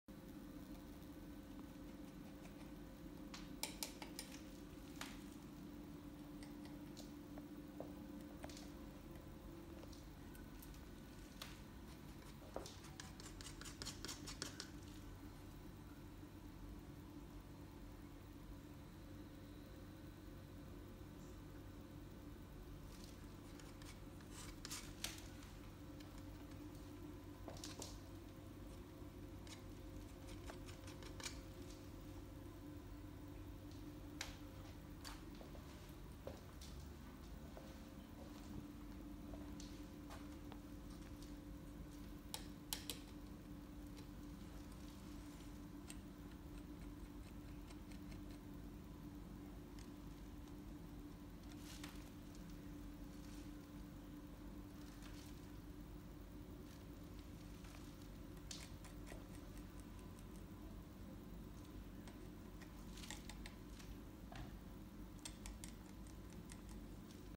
Lior KimmelArt, paintings and drawings
Oil painting on a slice of tree trunk of the City Safed. Painting with pallet knives